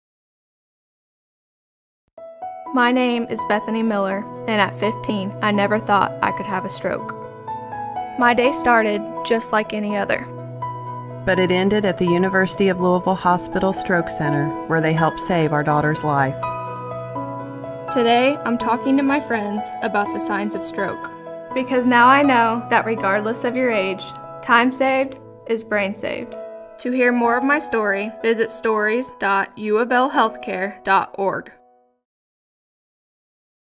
UL Bethany Miller - Teen Stroke Television Spot
University of Louisville Health Care Stroke Center patient, Bethany Miller.